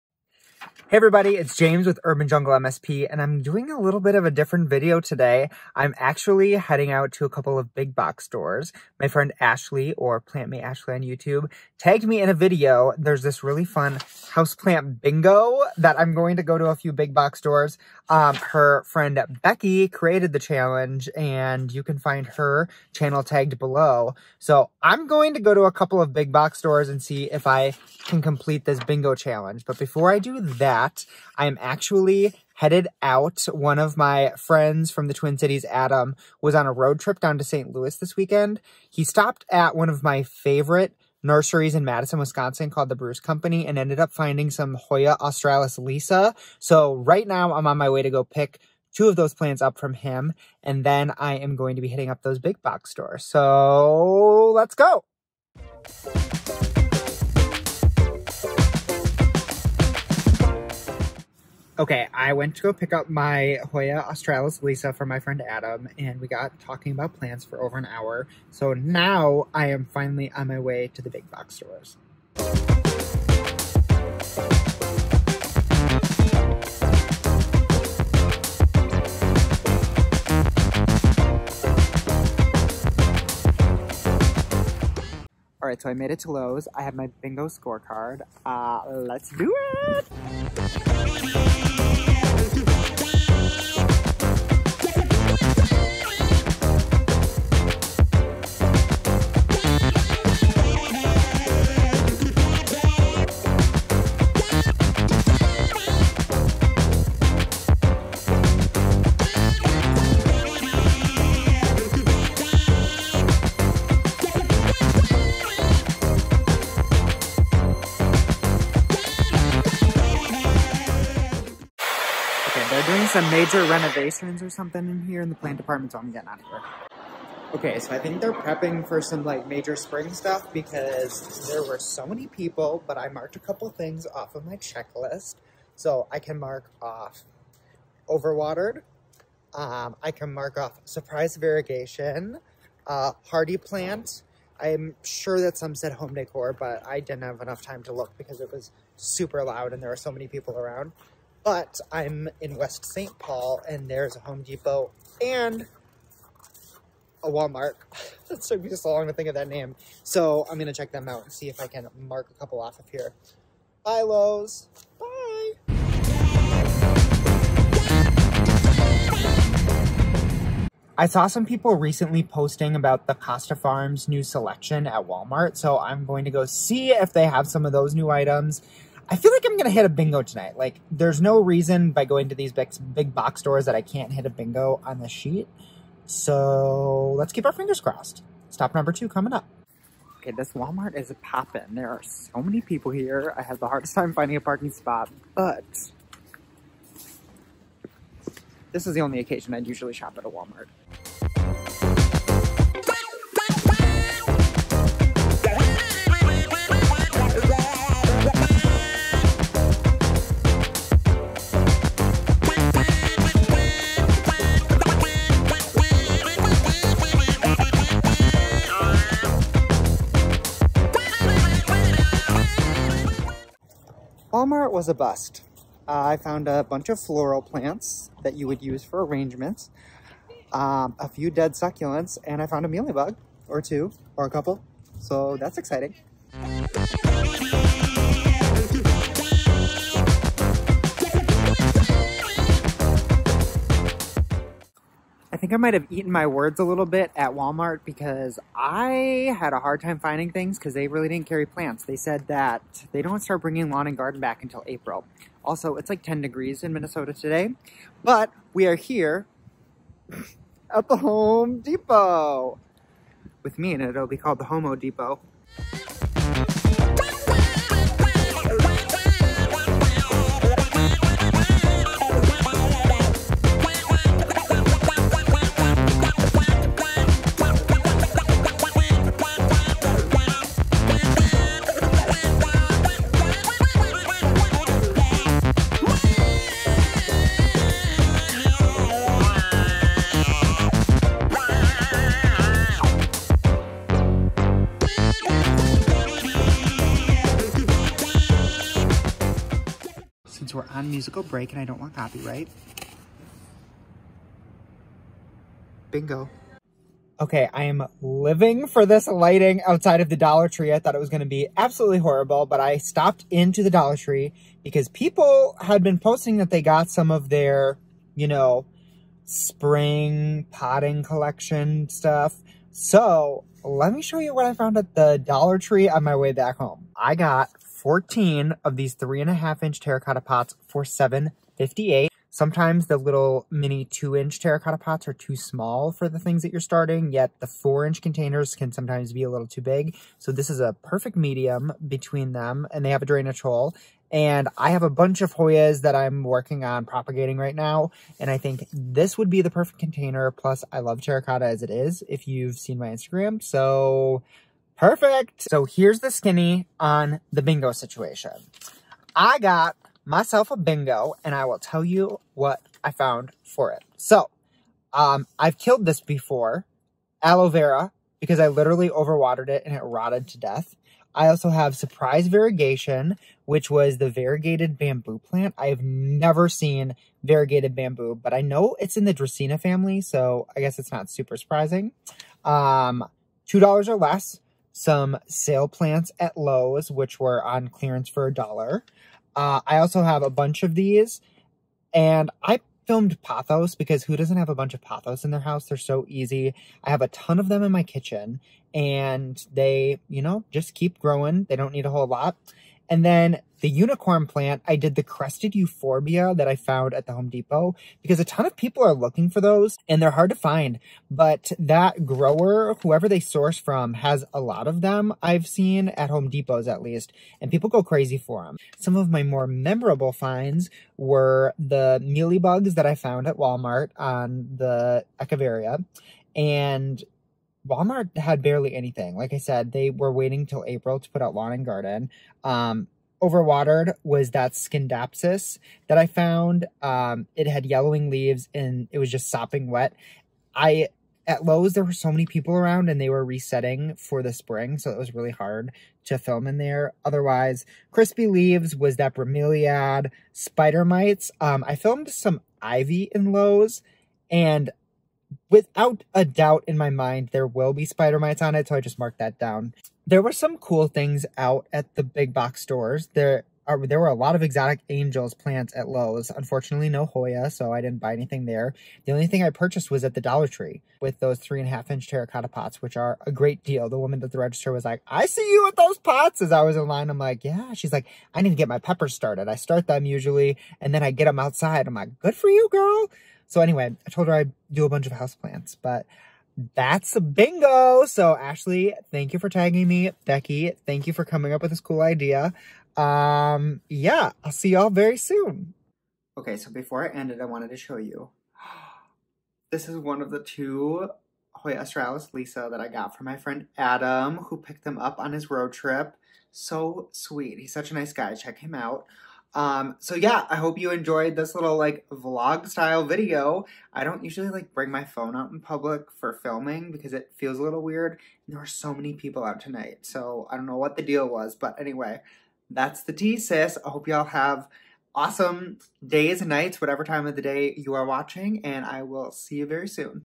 Big Box Houseplant Bingo and Wishlist Hoya australis 'Lisa'!!
I was tagged by my friend Plant Me Ashley to do Big Box Houseplant Bingo! I went out to Lowes, Walmart, and Home Depot to see what I could find. I also show you the awesome Hoya australis 'Lisa' my friend Adam picked up for me on his journey to St. Louis!